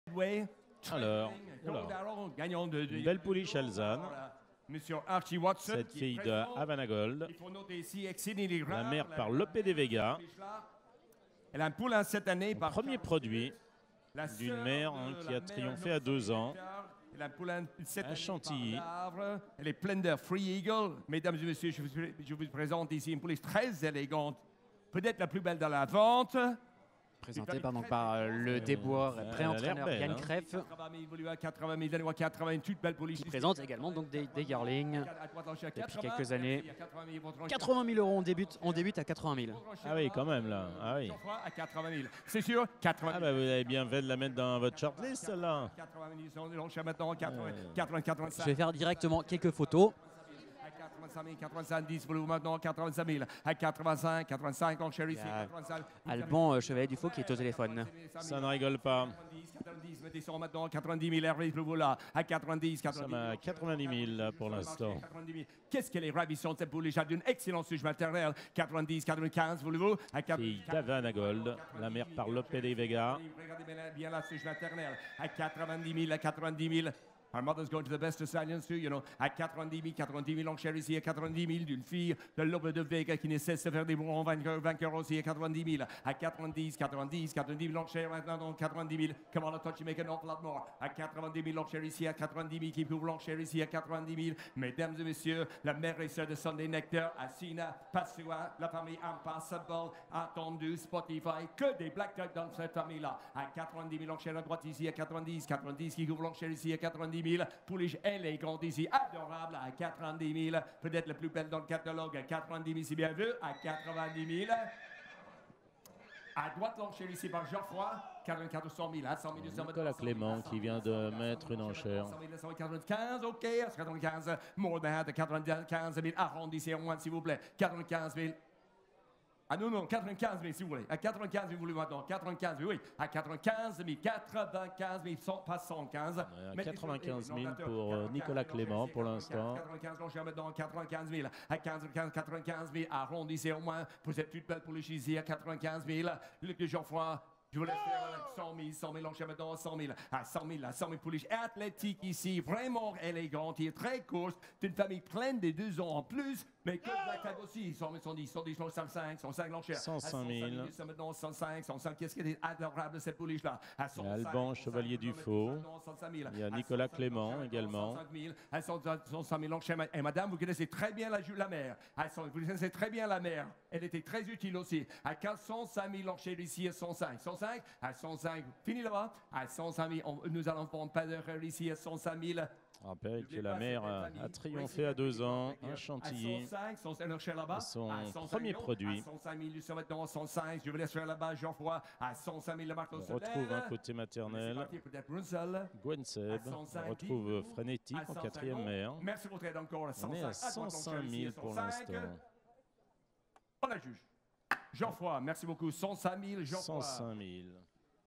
Top Price N°2 J2 - 105 000E - N (Exceedingly Rare 2017) - F17 - Vente Osarus de Yearlings 2018
Vente Osarus de Yearlings 2018 - La Teste de Buch
Top Price N°2 J2 - Lot 177
N (Exceedingly Rare 2017) - F17
105 000E - TINA RAU/ NICOLAS CLEMENT